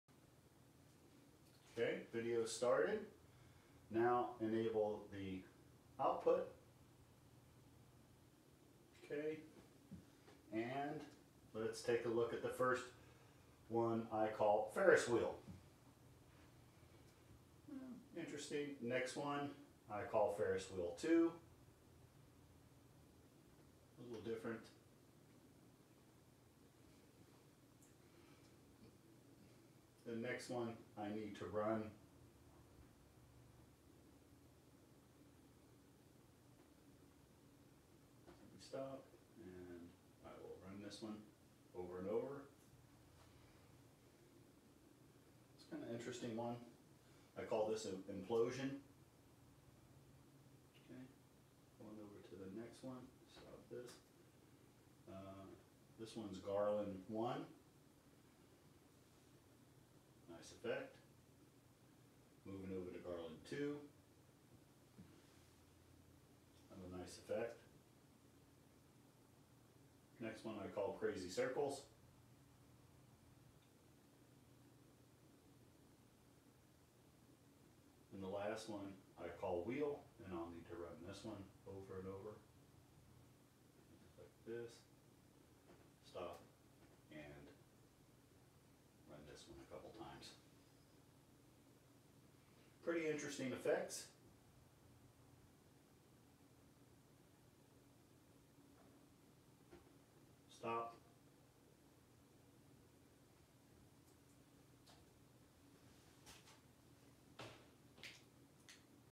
Coro Glow Spinnner
Check out how you can add a "Glow Ring" to your coro spinner to create a new prop called a "Glow Spinner".

Check out this video on how to build your own "Glow Spinner".